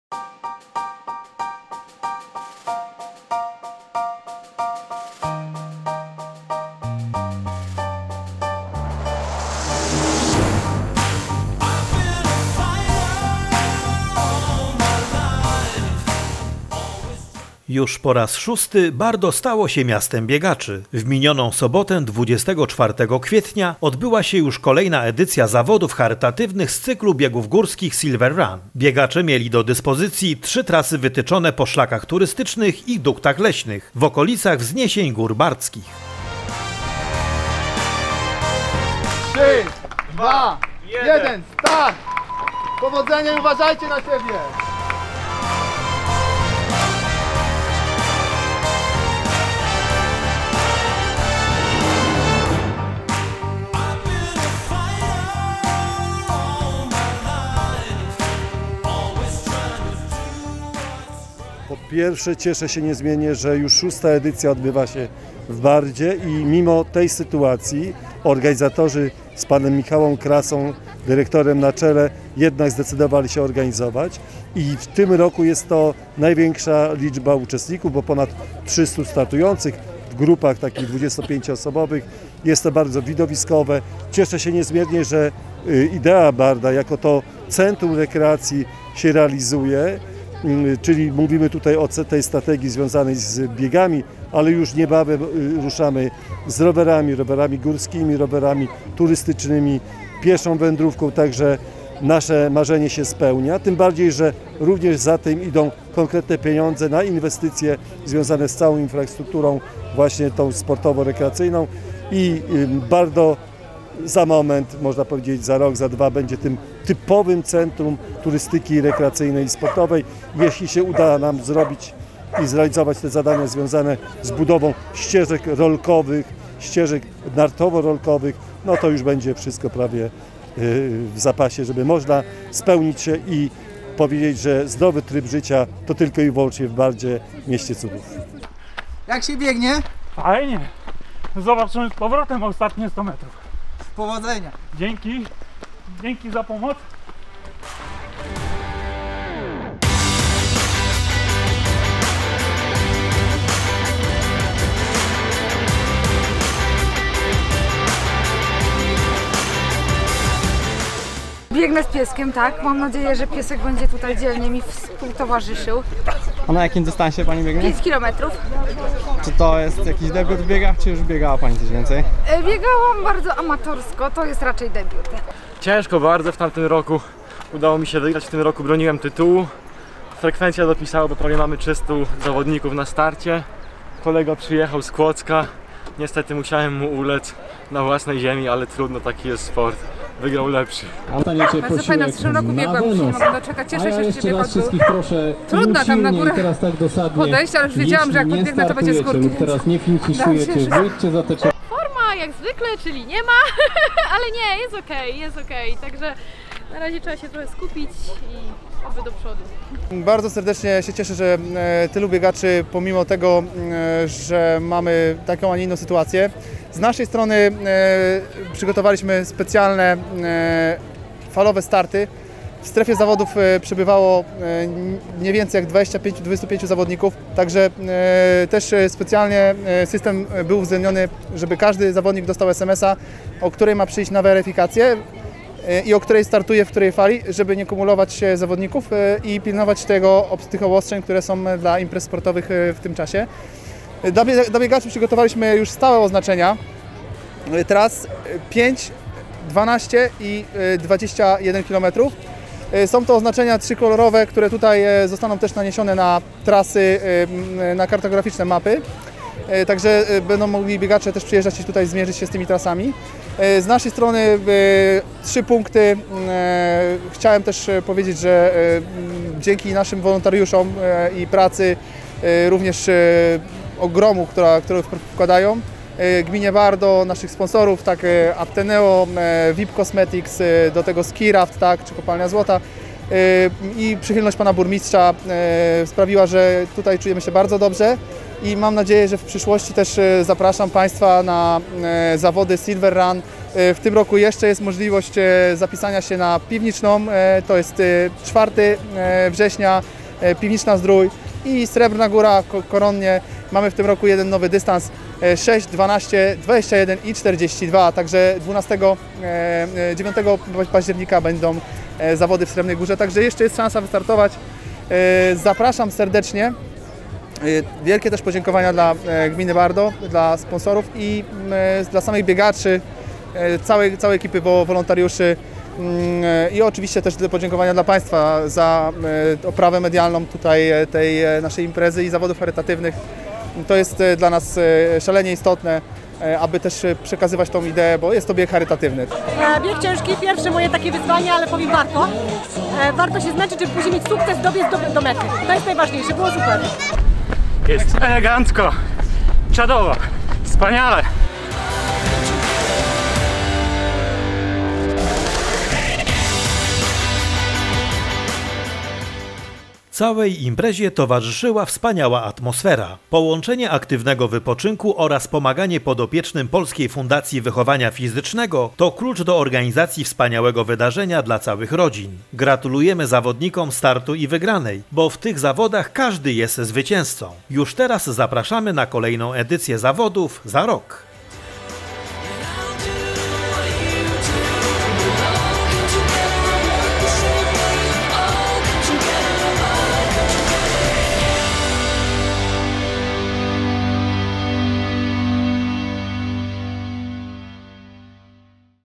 ZAWODY SILVER RUN - BIEGI GÓRSKIE W BARDZIE
Już po raz 6, Bardo zostało miastem biegaczy. W minioną sobotę 24 kwietnia odbyła się kolejna edycja zawodów charytatywnych z cyklu biegów górskich Silver Run. Biegacze mieli do dyspozycji trzy trasy wytyczone  po szlakach turystycznych i duktach leśnych w okolicach wzniesień Gór Bardzkich. Całej imprezie towarzyszyła wspaniała atmosfera. Połączenie aktywnego wypoczynku oraz pomaganie podopiecznym Polskiej Fundacji Wychowania Fizycznego, to klucz do organizacji wspaniałego wydarzenia dla całych rodzin. Gratulujemy zawodnikom startu i wygranej, bo w tych zawodach każdy jest zwycięzcą. Już teraz zapraszamy na kolejną edycję zawodów już za rok.